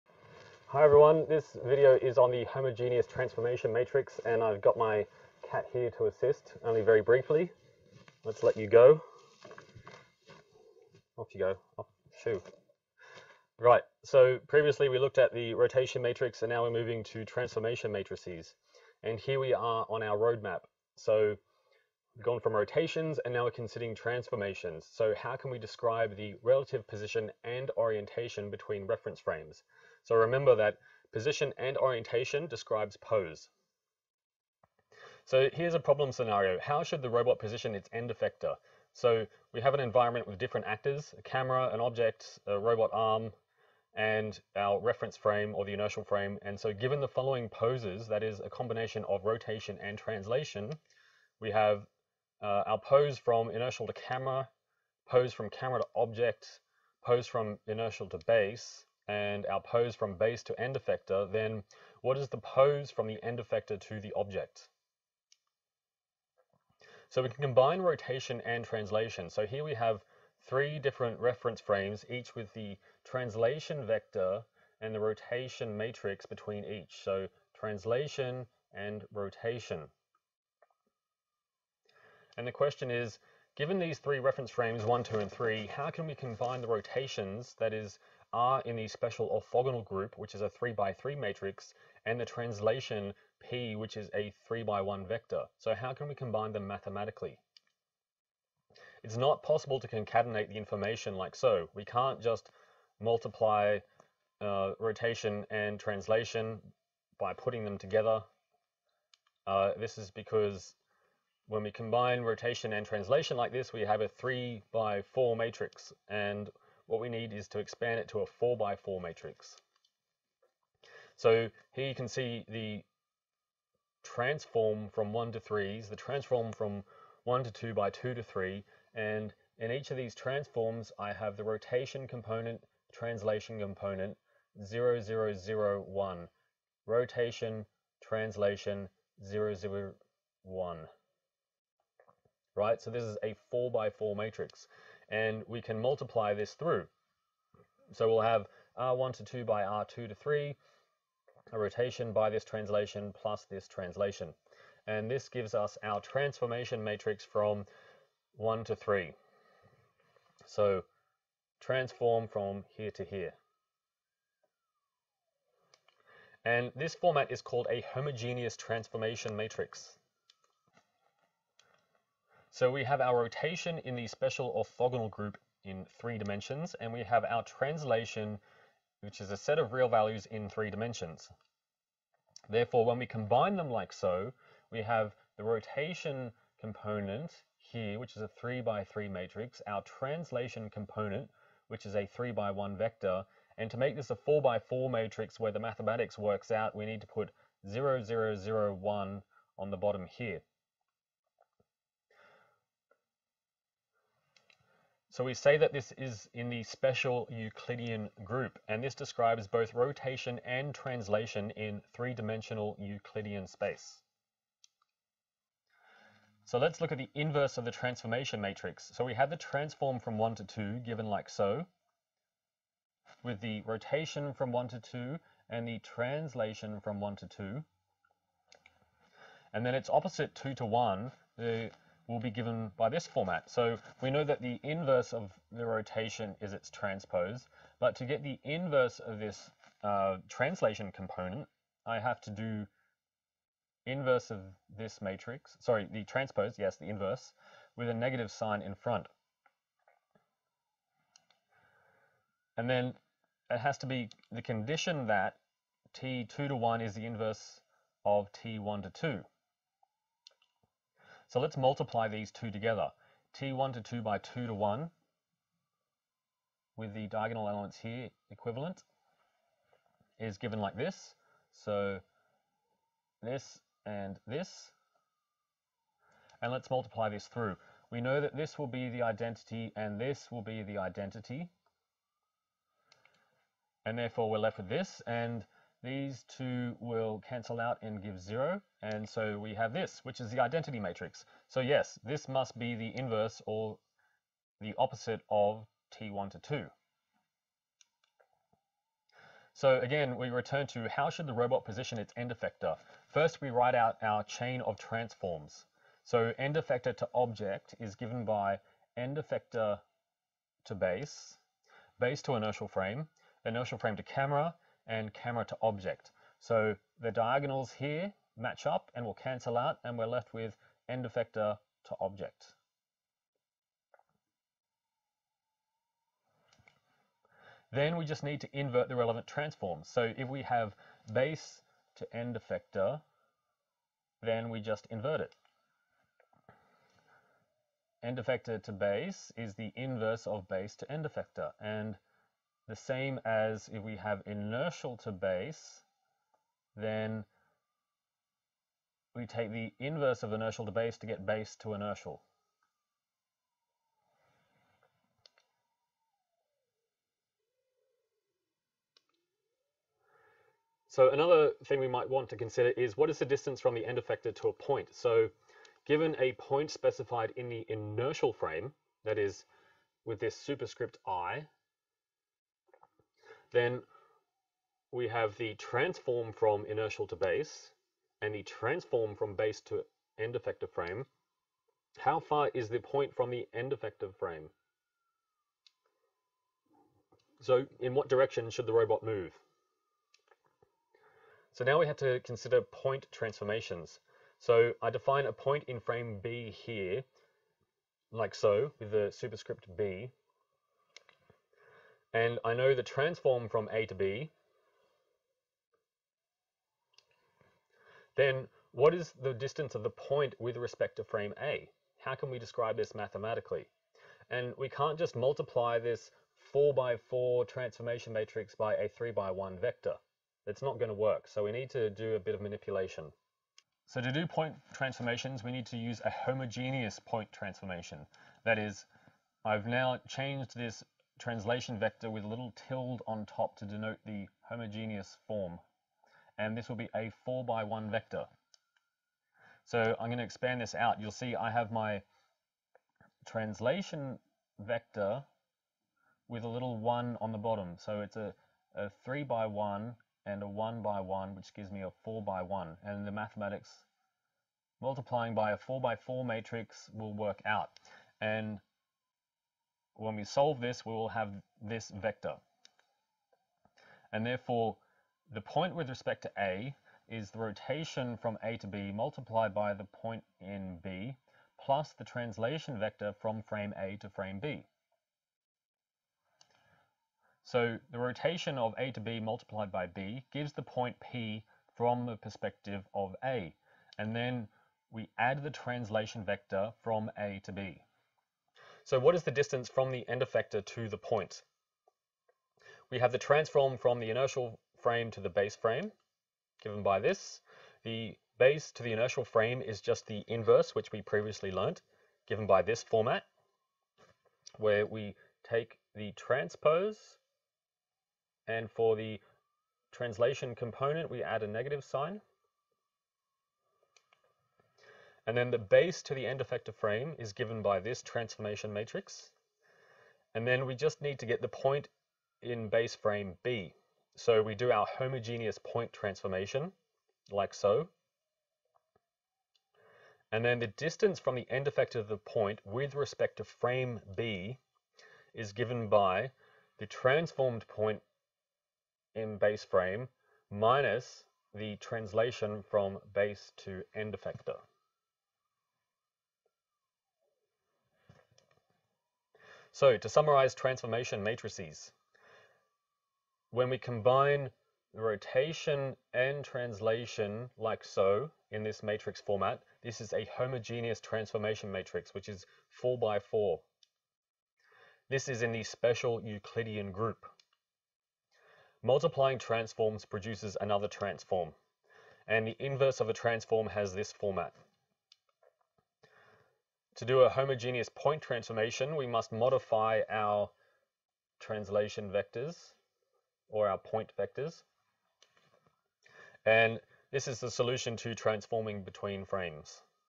3.1 Transformation Matrices
In this lecture, I introduce the concept of a transformation matrix which combines a rotation matrix in the Special Orthogonal group SO(3), and a translation vector in 3D Euclidean space.  I develop the inverse of a transformation matrix, show how to perform point transformations between frames, and chain transformation matrices to find the relative transform between reference frames in a given scenario.